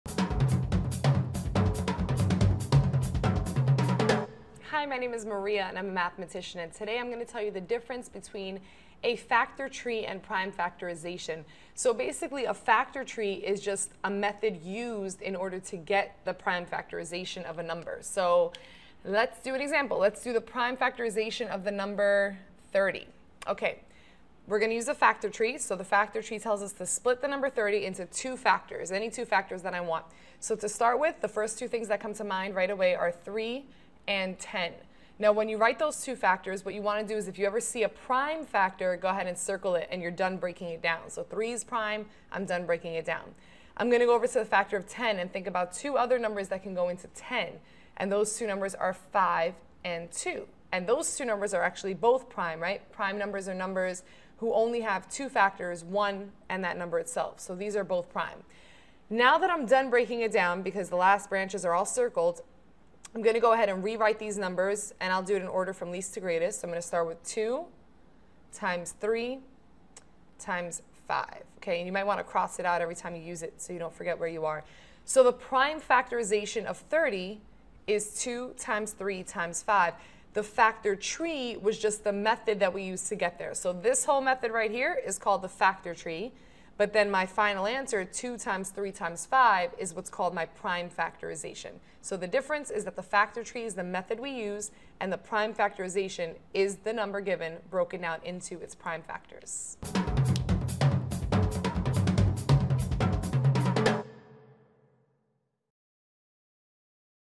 What Is the Difference Between a Factor Tree & Prime Factorization?
What Is the Difference Between a Factor Tree & Prime Factorization?. Part of the series: Algebra Help. A factor tree and prime factorization are similar concepts but are different in key ways. Learn about the differences between factor trees and prime factorization with help from a mathematician and educator in this free video clip.